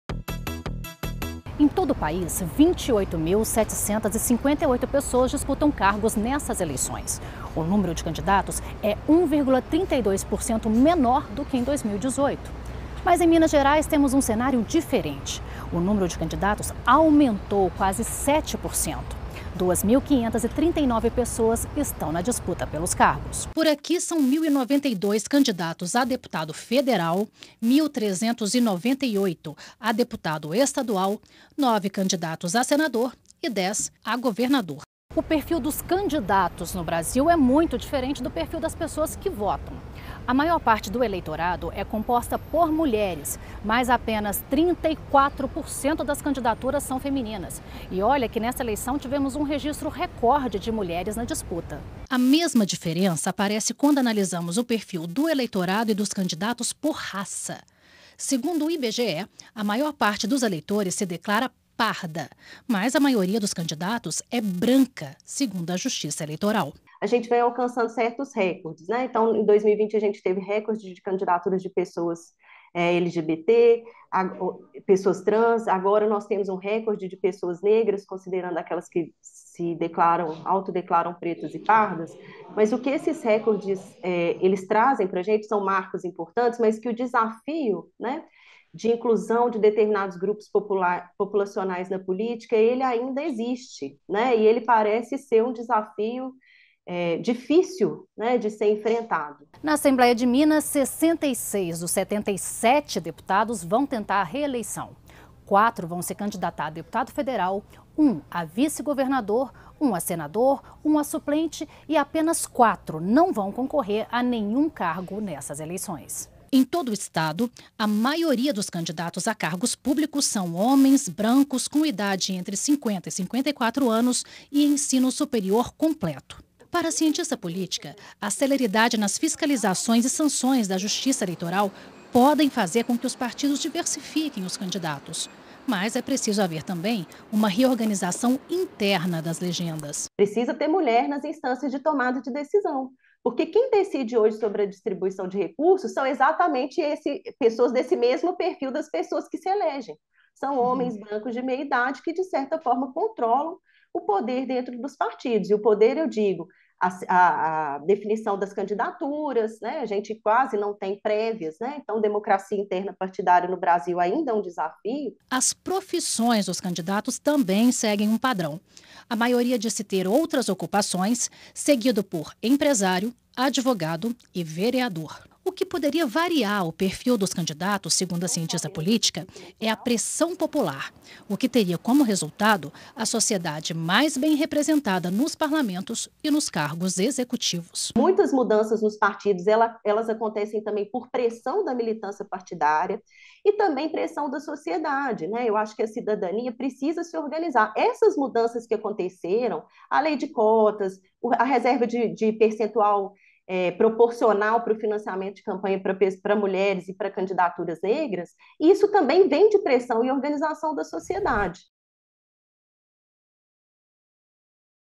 Conheça o perfil médio dos candidatos em 2022
Os candidato para as eleições de 2022 em todo o país são, em sua maioria, homens, casados e com curso superior completo. O levantamento, feito pelo Tribunal Superior Eleitoral, traz dados nacionais e também por estados. Em Minas, o perfil dos candidatos segue esse padrão nacional. São 2.539 pessoas na disputa por cargos; a maioria homens (67%); brancos (48.96%); com idade entre 50 e 54 anos (15,91%) e com ensino superior completo (50,37%).

A cientista política da UFOP, Universidade Federal de Ouro Preto, Daniela Rezende, analisa os números e acredita que esse perfil de candidatos ainda não representa a sociedade como um todo. Daniela também afirma que seria preciso aumentar a participação das minorias no pleito. Segundo ela, a celeridade nas fiscalizações e a celeridade nas sanções da Justiça Eleitoral junto aos partidos políticos são medidas que podem surtir efeito, além da reorganização interna das legendas e da pressão da sociedade.